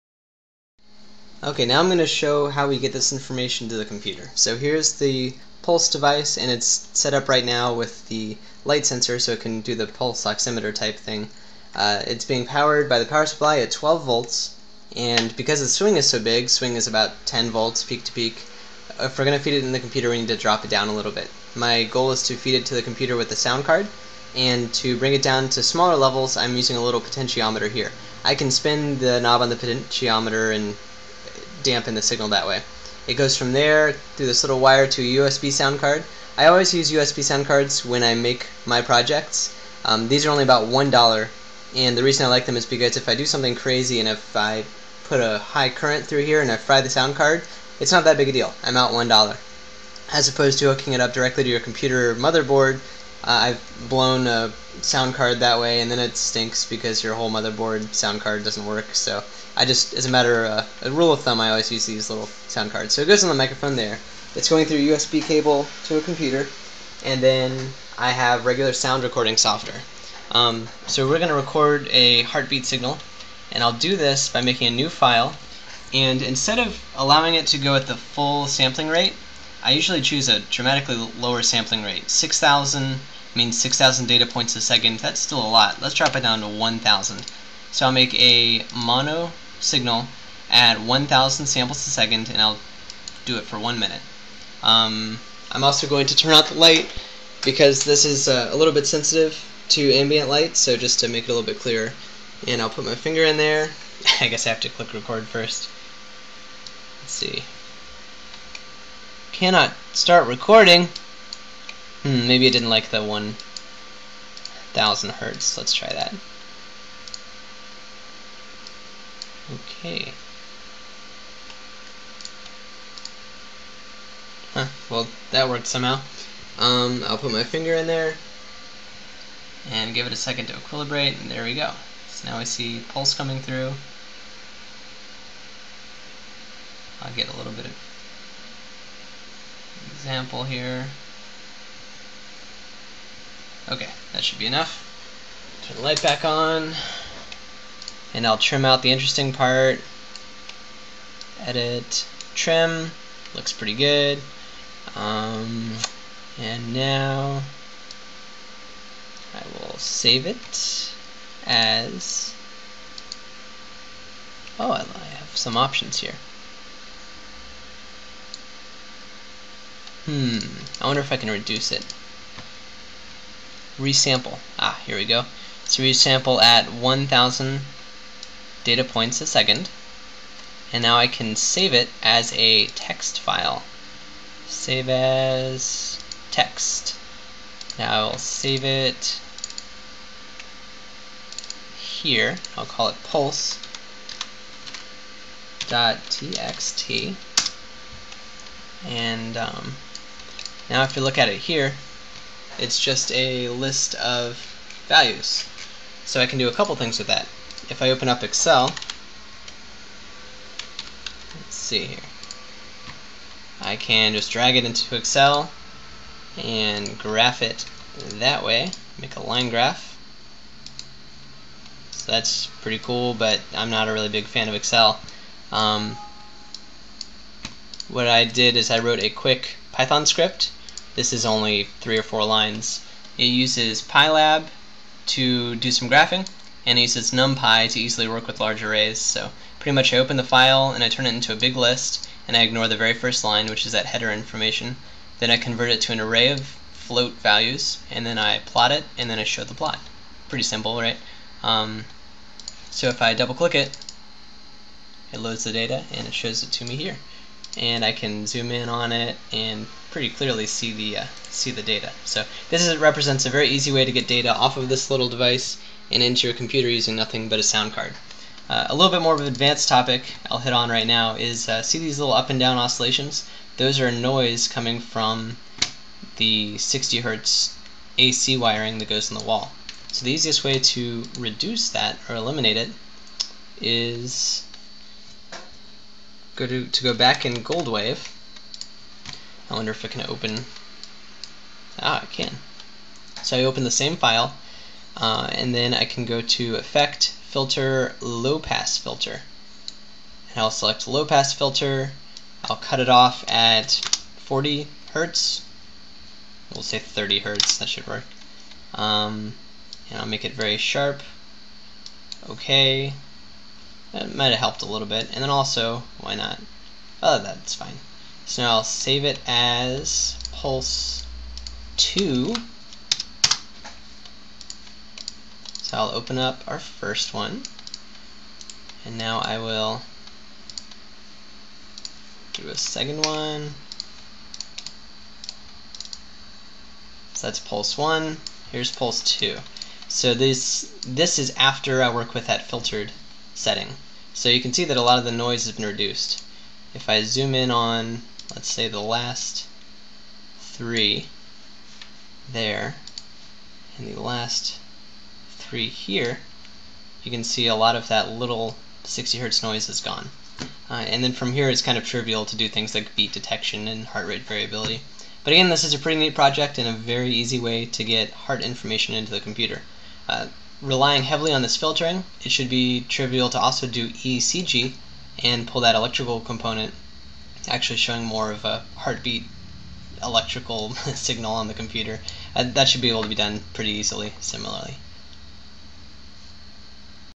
DIY ECG and Pulse Oximeter with LM324 OpAmp (2 of 2)
This is a smaller part of a bigger project. It's a single LM324 quad operational amplifier using all 4 amps: (1) virtual ground, (2) preamplifier, (3) lowpass filter, (4) final amplifier and second lowpass filter. It doubles as a pulse oximeter (near rail-to-rail swing at 12V and 5V) with a phototransistor, and an ECG (about 0.5 V output).